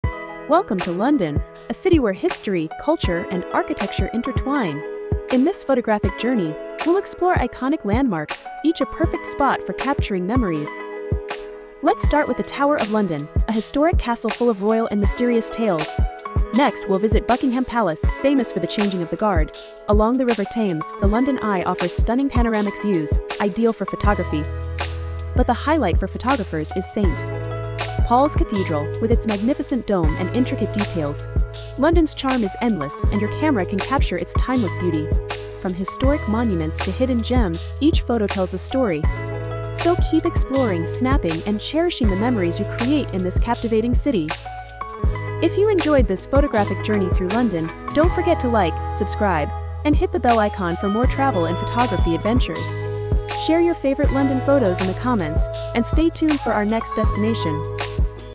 Exploring London's Iconic Sights A Photographic Journey
Join us on a captivating photographic journey through London, a city brimming with history, culture, and stunning architecture. From the historic Tower of London to the iconic Buckingham Palace and the breathtaking views from the London Eye, we'll guide you through the best spots to capture the essence of this vibrant city. Whether you're a photography enthusiast or simply looking to explore London's famous landmarks, this video is your ultimate guide.